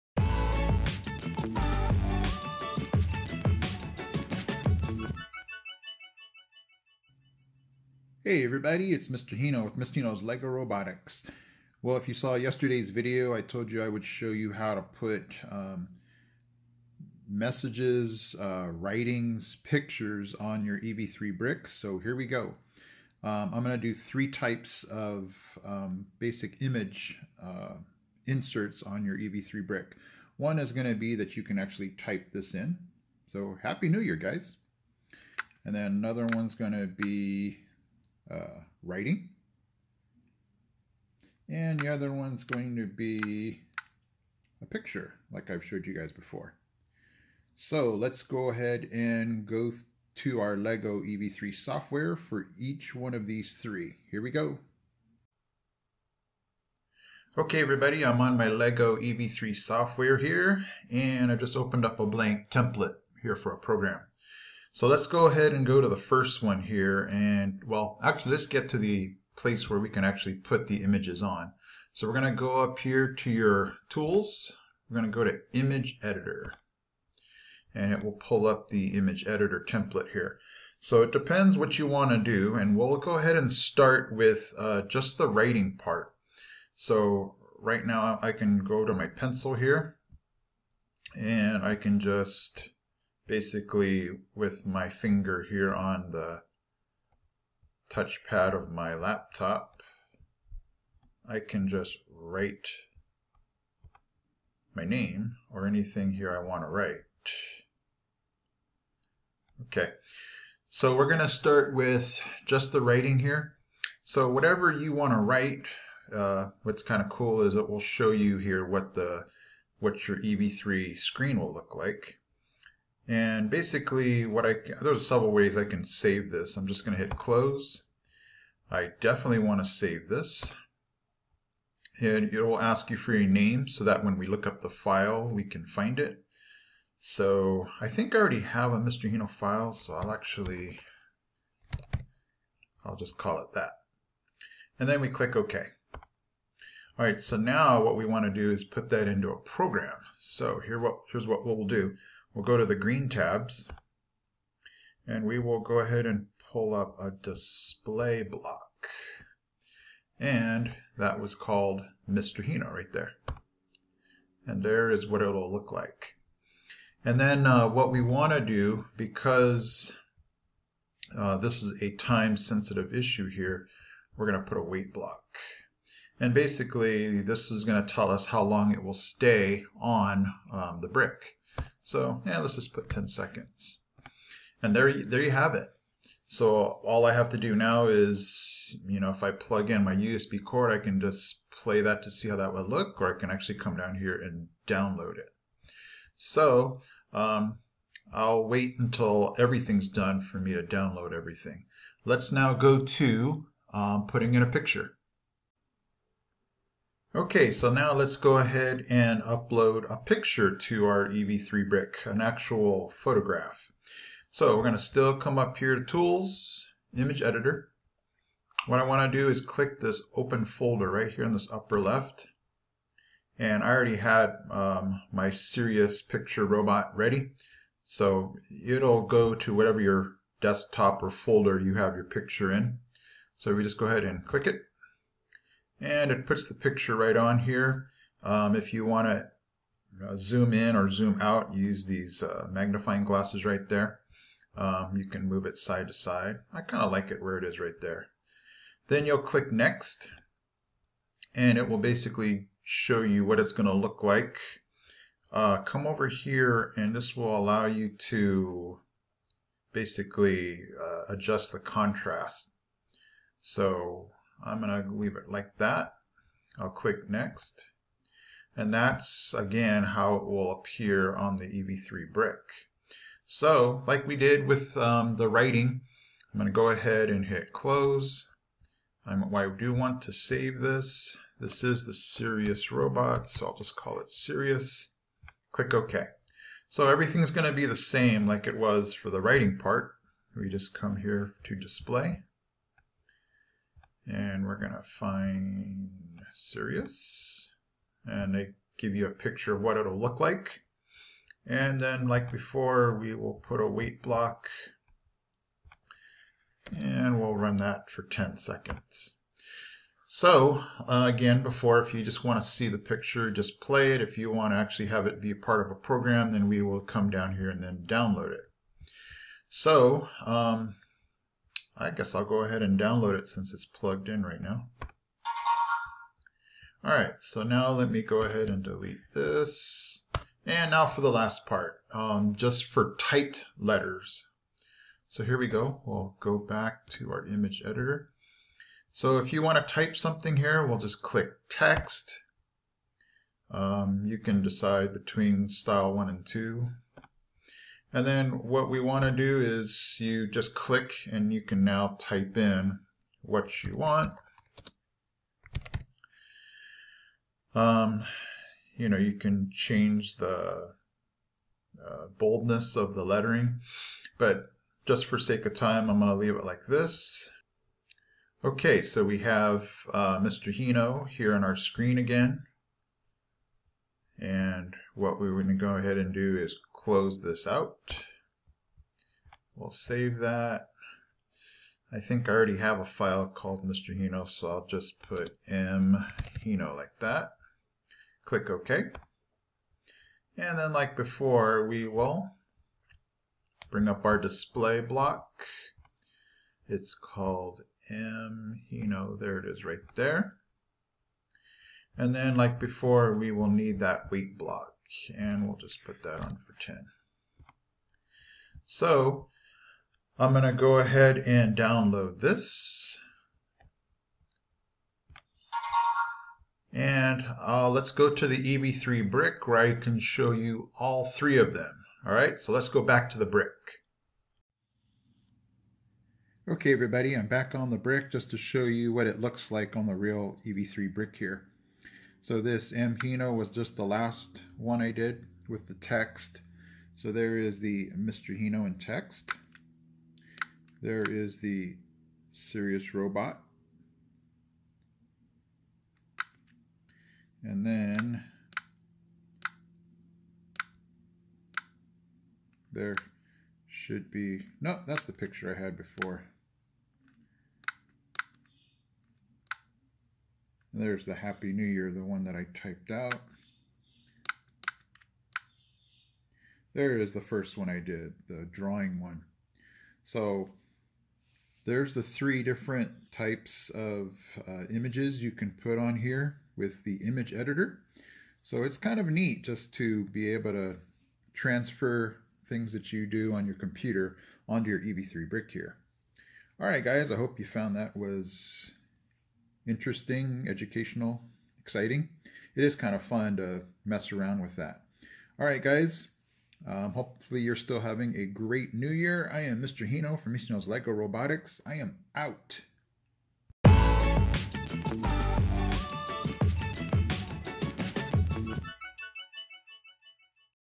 "LEGO EV3: How To Put Images On Your LEGO EV3 Brick Using Image Editor"
Hey guys!!  This video will show you how to download images from pictures, drawings, and type to your LEGO EV3 Brick using the Image Editor on the LEGO EV3 software.  It's really fun and easy to do!!  Hope you enjoy!!